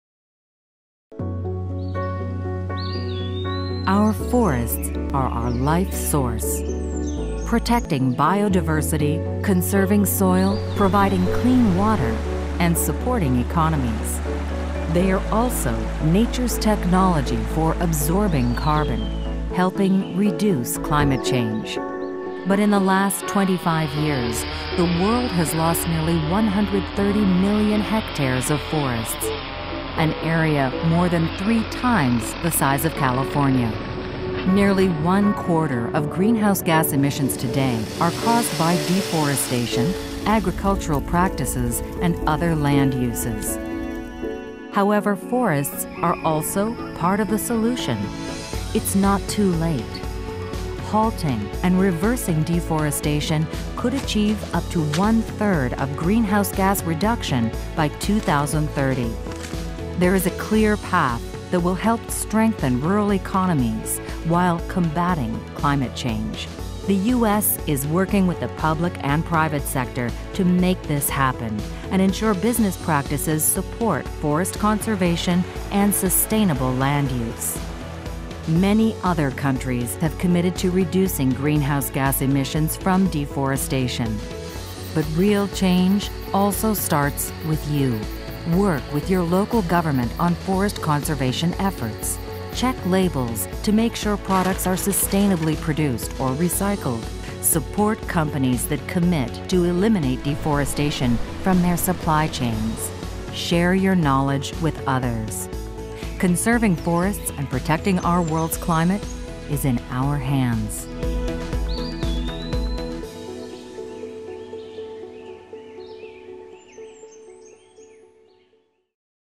Solusi Untuk Perubahan Iklim - Apa yang Bisa Kamu Lakukan?
Sudah bukan rahasia lagi bahwa hutan merupakan sumber kehidupan bagi kita dan makhluk hidup lainnya. Namun sayangnya, dalam 25 tahun terakhir, jumlah area hutan semakin banyak berkurang. Apa yang bisa kita lakukan untuk membantu melestarikan hutan? Cari tahu lebih lanjut cara mengurangi jumlah emisi karbon di sini